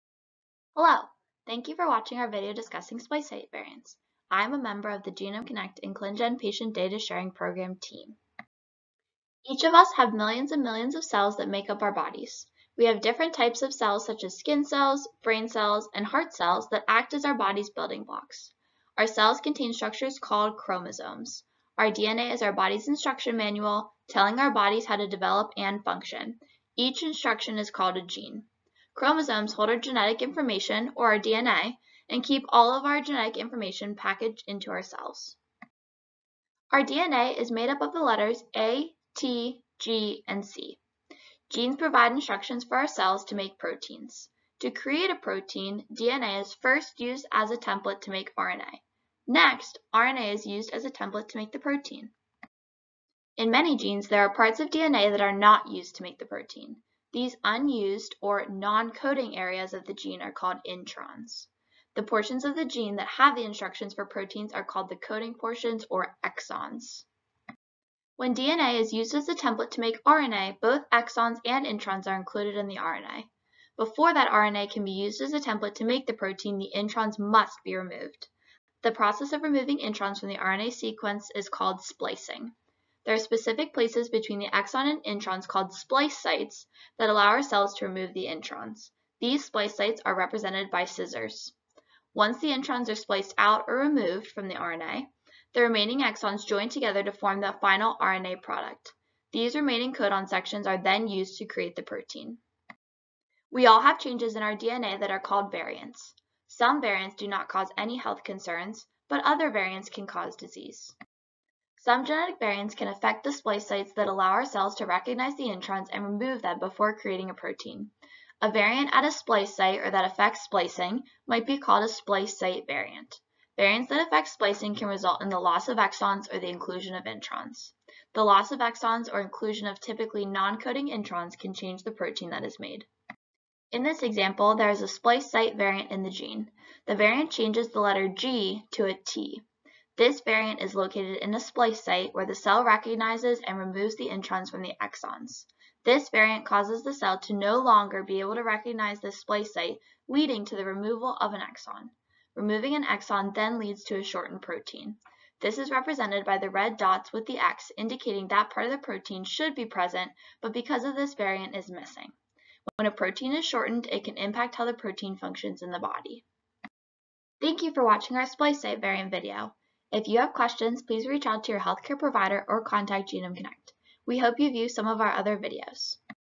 Types of Sequence Variants - Splice Site Variants
This video provides an overview of splice site variants. When DNA is used as a template to make RNA, both exons and introns are included in the RNA. Before RNA can be used as a template to make the protein, the introns must be removed. The process of removing introns from the RNA sequence is called “splicing.” When a variant occurs in this region, it is called a splicing variant.